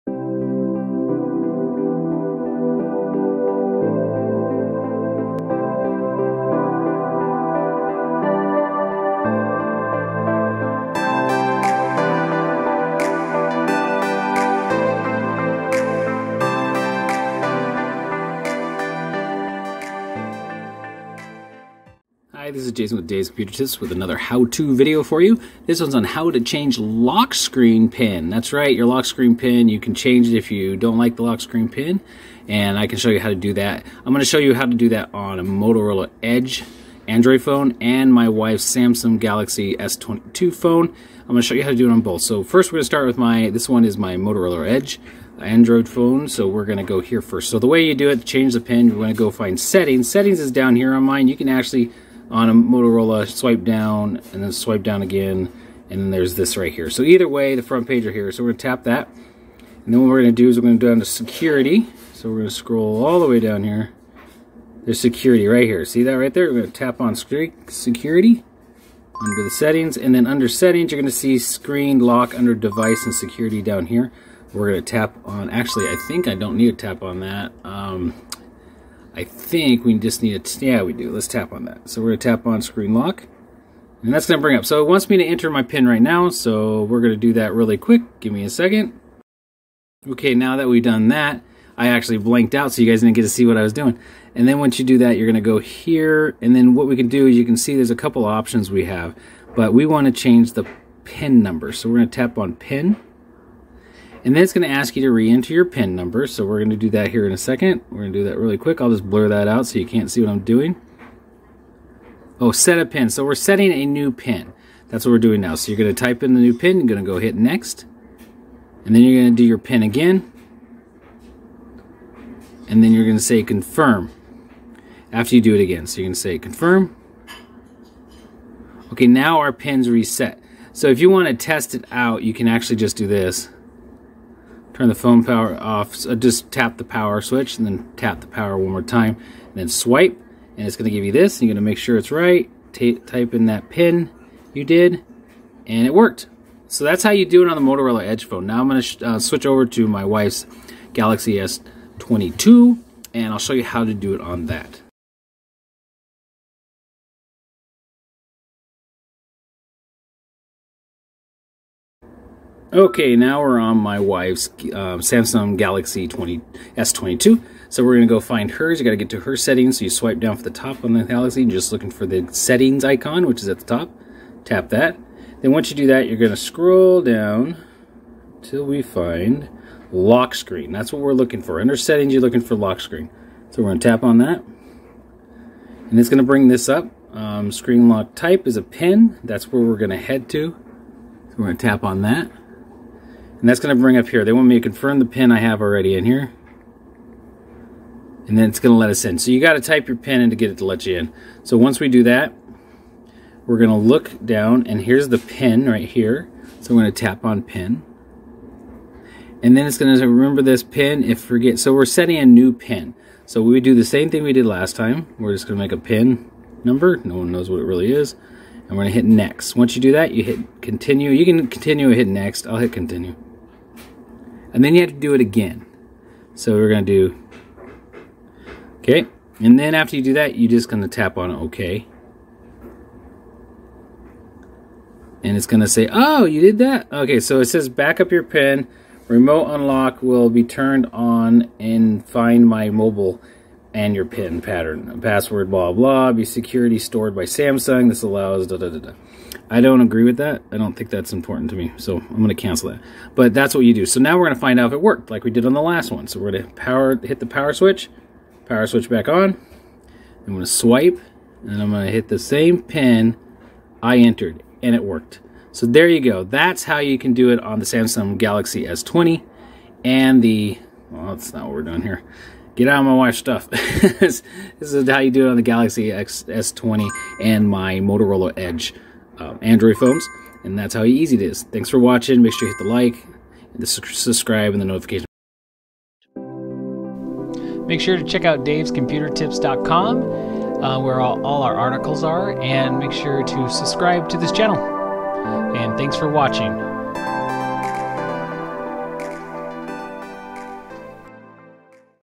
How To Change Lock Screen PIN
In this video I will be showing you how to change your lock screen PIN. This will be shown on my Motorola Edge and a Samsung Galaxy S22 android phones.

Many of us own a smartphone and most likely use a PIN to unlock it. It is important to keep others from getting your personal information or reading your text messages. The other day I was playing around on my phone and thought it might be a good time to change my PIN to log in to my phone. As with any security feature, it is always good to update it every so often. In this article, I want to show you how to update this and make sure that it stays secure. Before we move on, make sure to check out the other videos I have created on Android devices:

Guess it is time to get this lesson started.